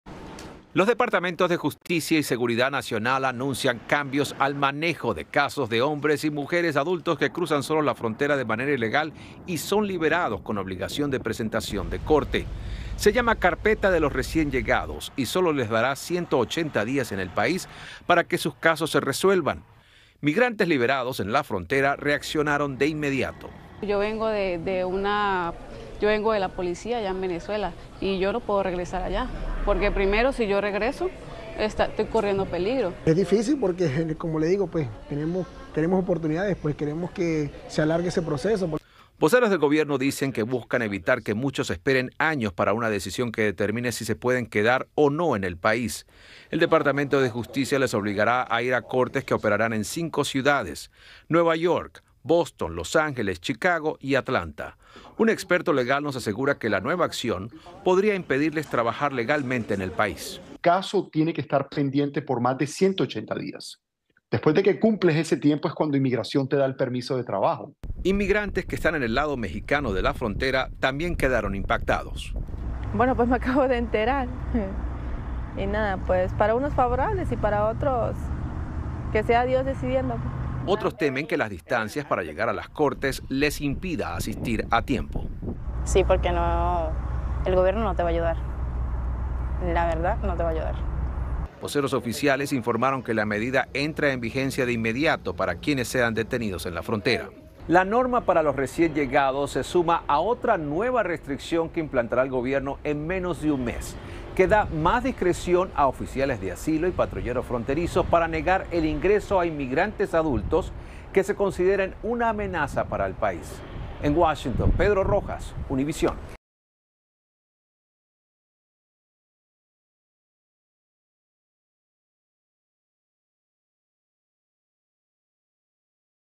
Las claves de los nuevos cambios a la política de asilo anunciados por el gobierno Biden
Los departamentos de Justicia y Seguridad Nacional anunciaron este jueves cambios al manejo de casos de mujeres y hombres adultos que cruzan solos la frontera de manera irregular y que son liberados con obligación de presentación de corte. Según se informó, a estas personas se les obligará a ir a cortes que operarán en cinco ciudades. Además, solo tendrán 180 días en el país para que sus casos se resuelvan.

Maity Interiano